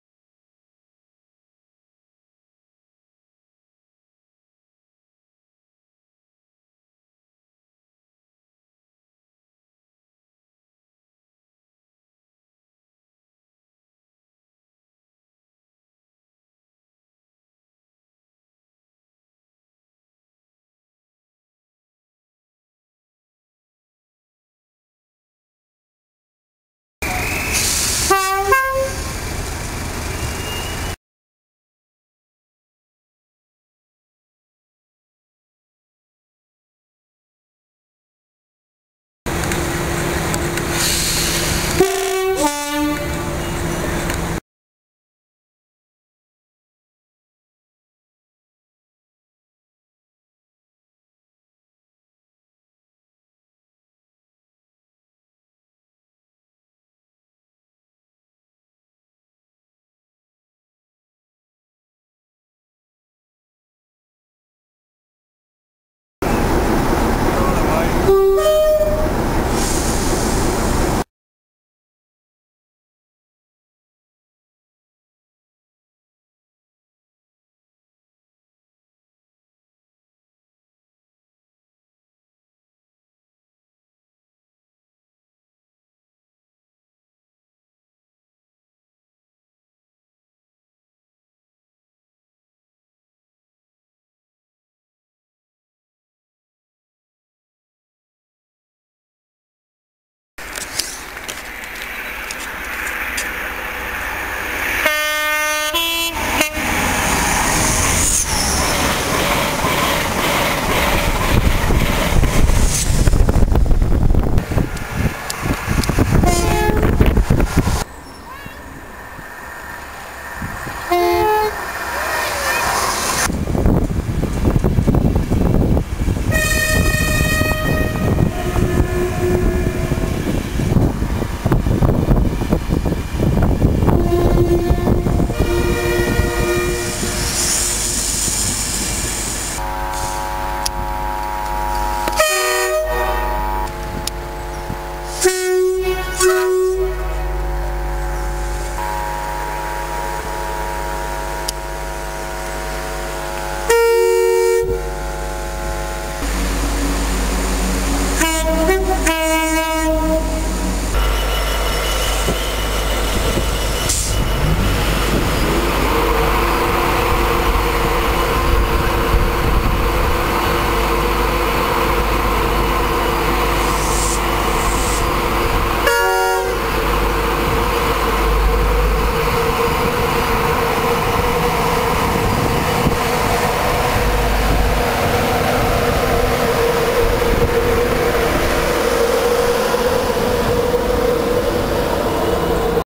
cab pics and horns
loco cabs and some unseen horns near the end!!